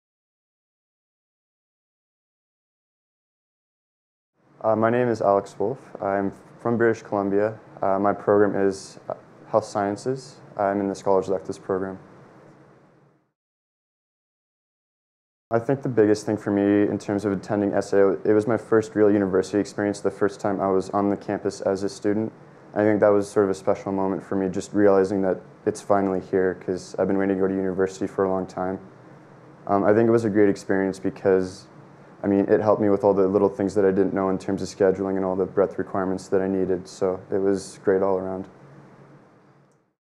Why is Summer Academic Orientation (SAO) important?
Discussing how Summer Academic Orientation (SAO) will get you off to a successful start at Western University.